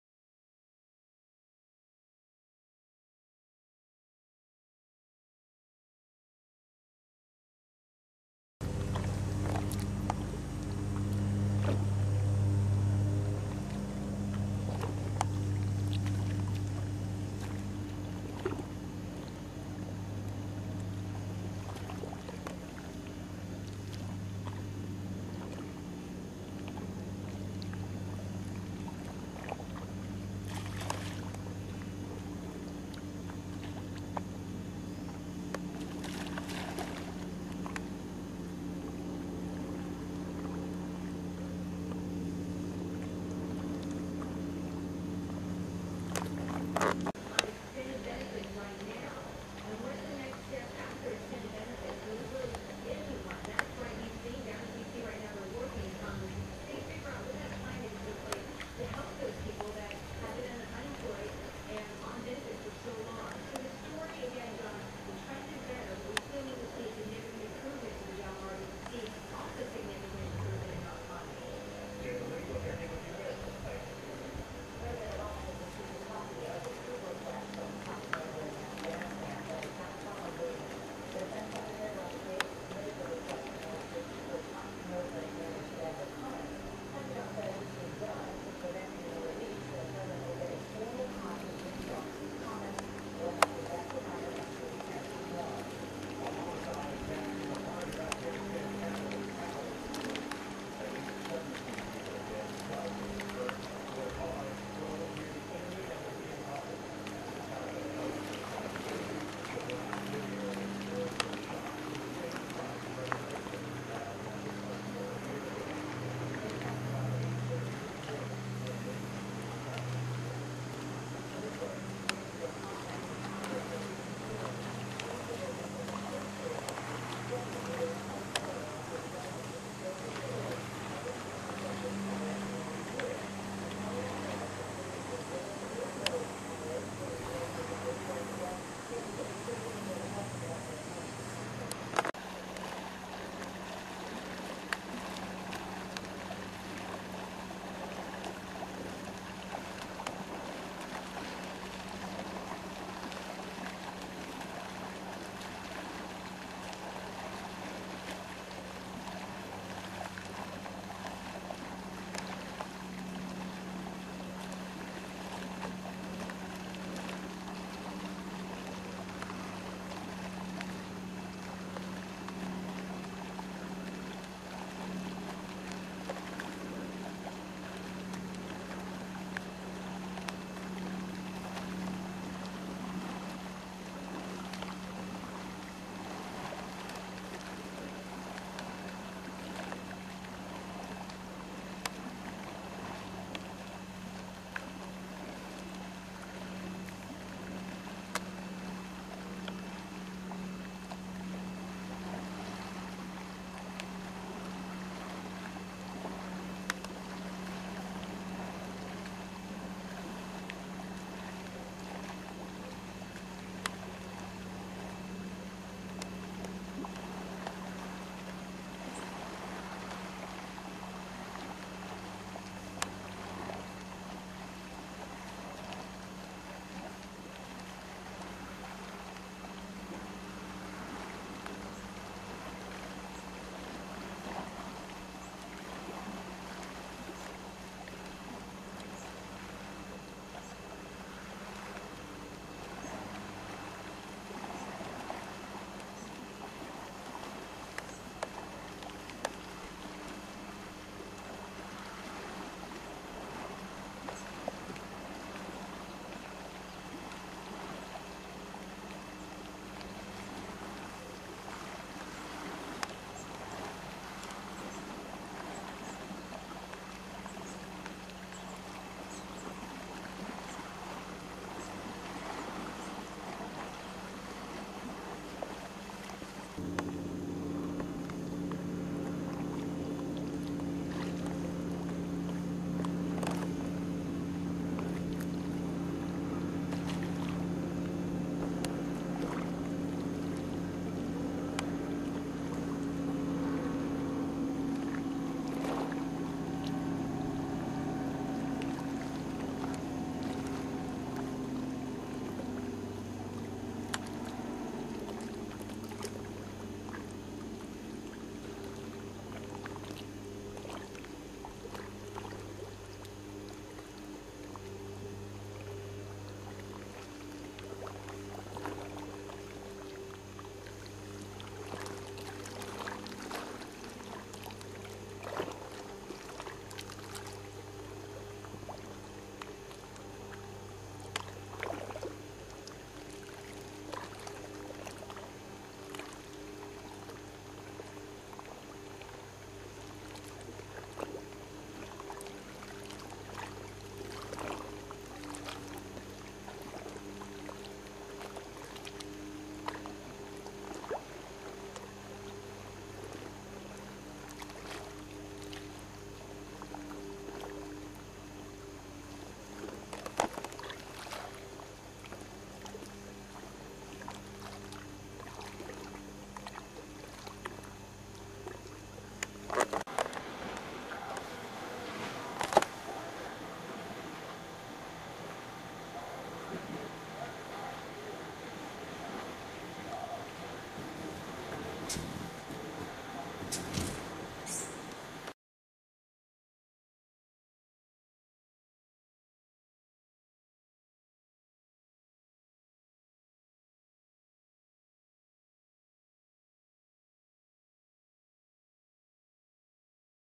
Fish Feeding Frenzy Off our Vacation Rental in Great Guana Cay, Abacos, The Bahamas
FISH ATTACK!  FISH FEEDING FRENZY off the shore in the Bahamas, in Great Guana Cay, Abacos.